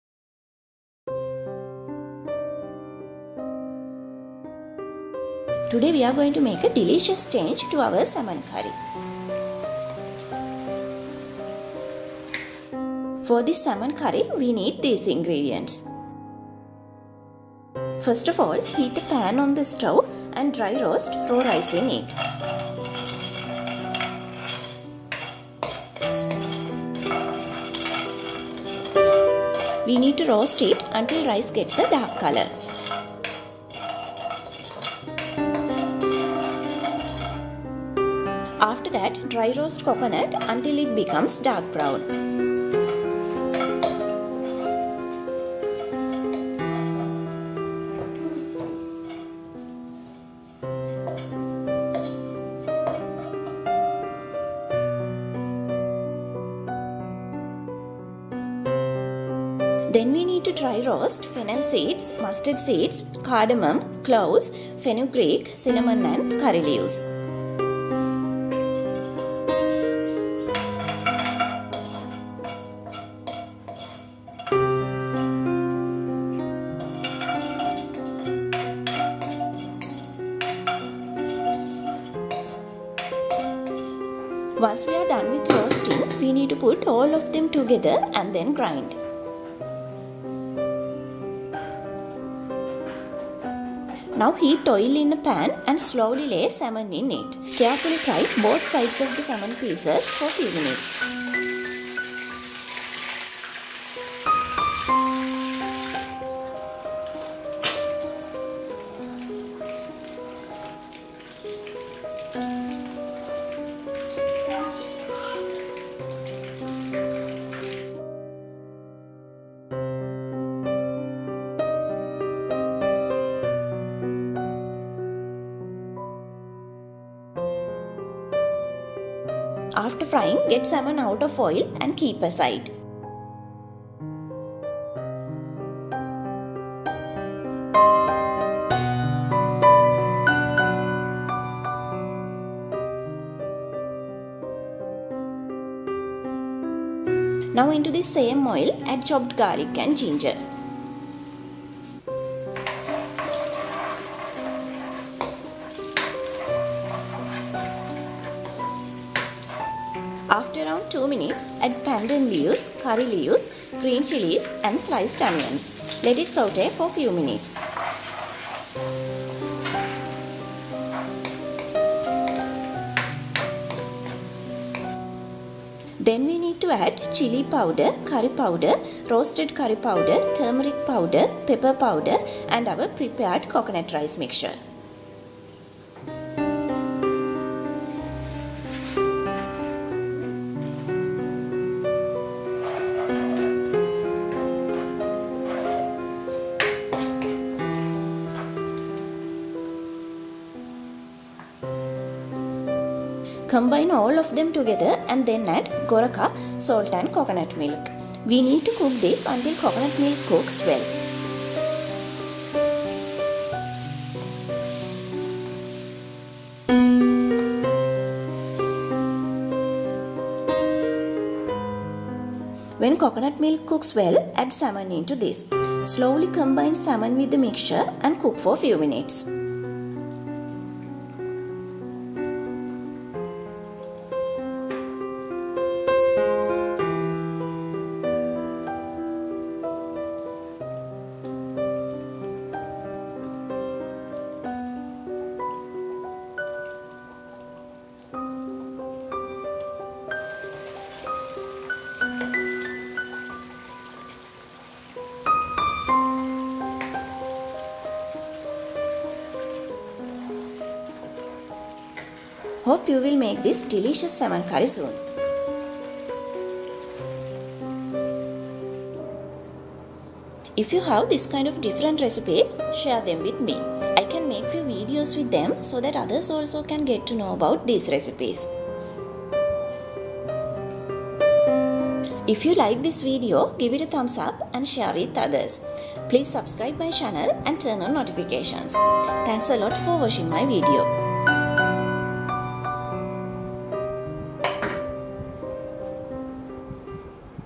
Salmon Kalu Pol Curry Recipe - English
This is a delicious, bit different way of cooking salmon.